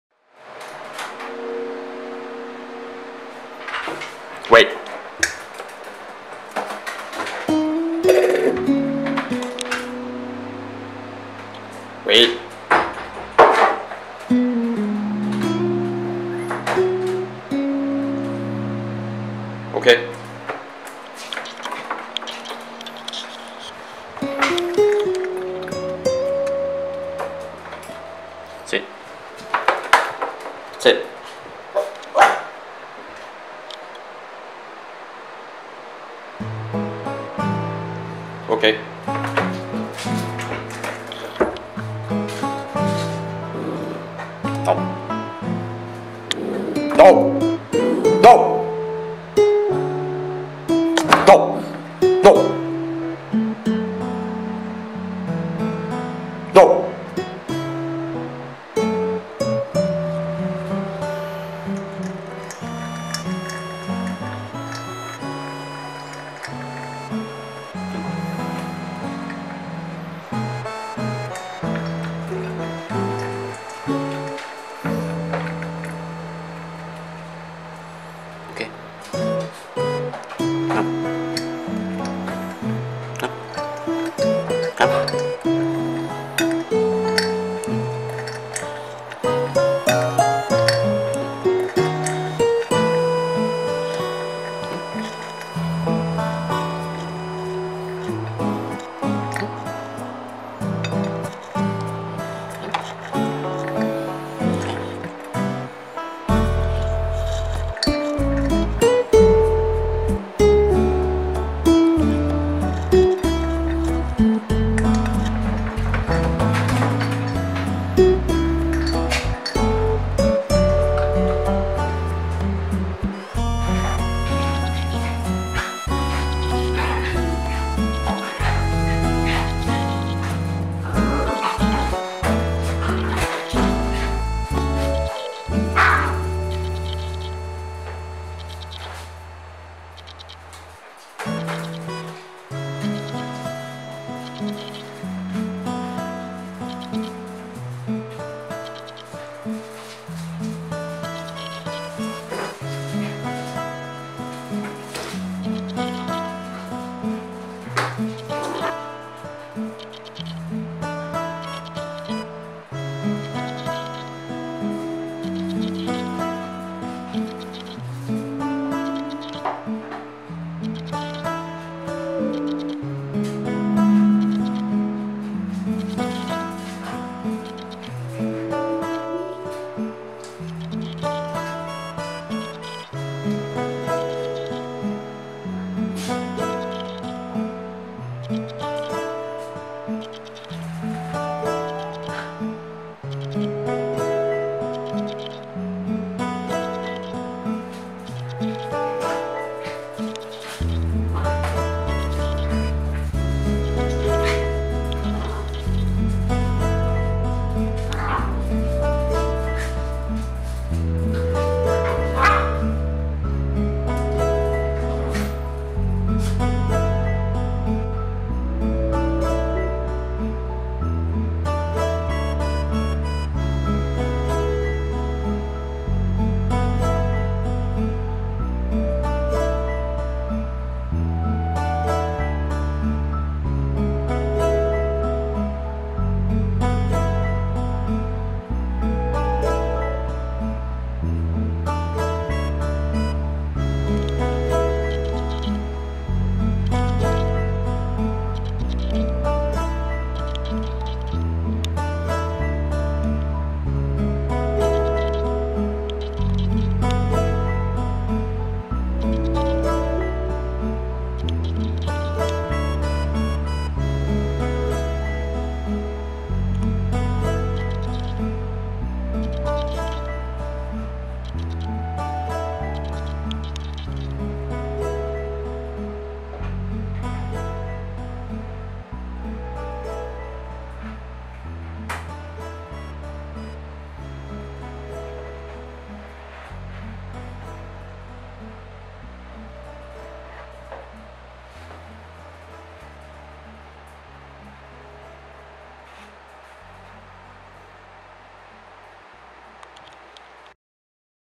Toy Poodles - Bubbles, Gin & Melon
Pretty stable and expected improvements on Melon's food aggression.